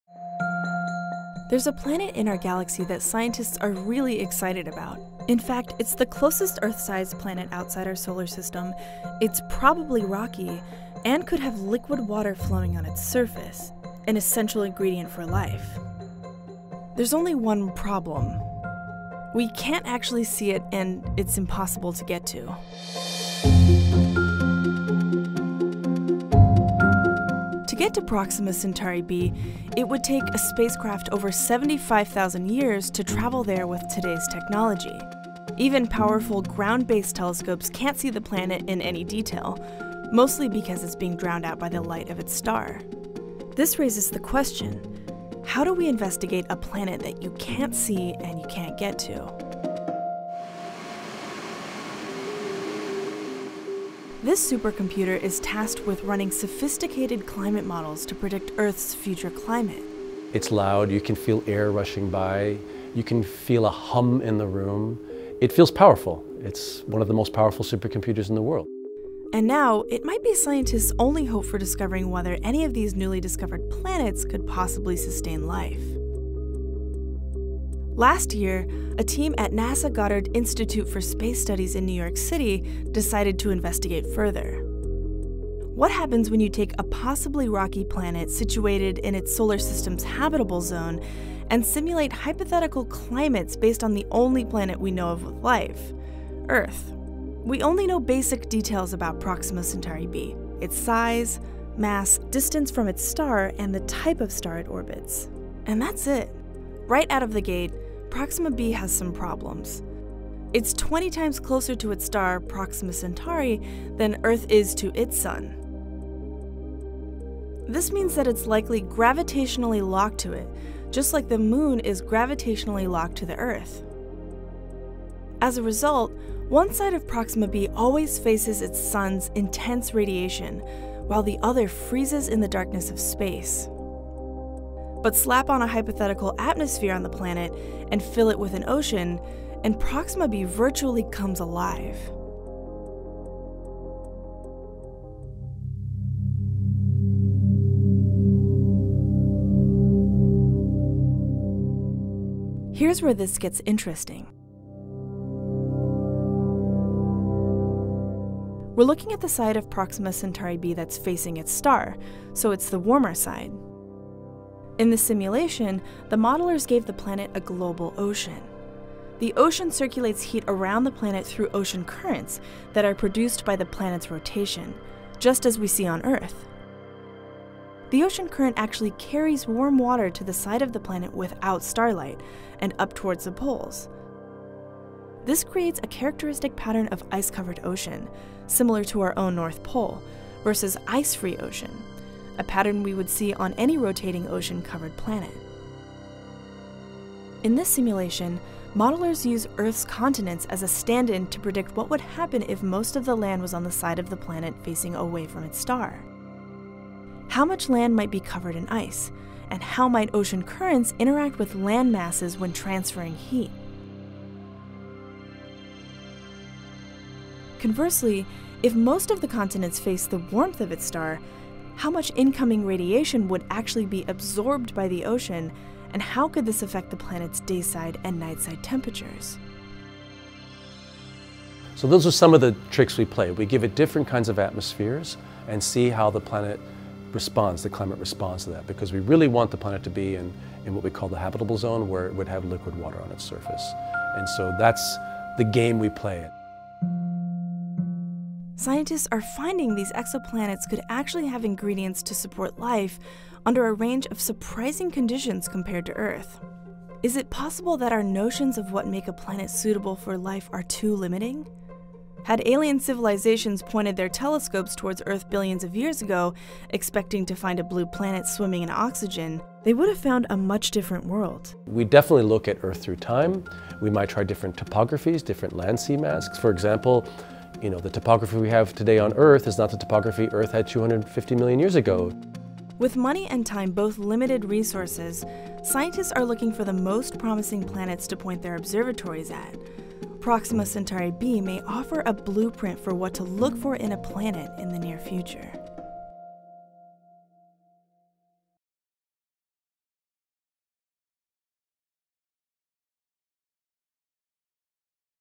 Earth Climate Models Bring Exoplanet To Life
In a generic brick building on the northwestern edge of NASA’s Goddard Space Flight Center campus in Greenbelt, Md., thousands of computers packed in racks the size of vending machines hum in a deafening chorus of data crunching. Day and night, they spit out five quadrillion calculations per second. Known collectively as the Discover supercomputer, these machines are tasked with running sophisticated climate models to predict Earth’s future climate. But now, they’re also sussing out something much farther away: whether any of the more than 4,000 curiously weird planets beyond our solar system — or exoplanets — discovered in the past two decades could have the ingredients necessary to support life.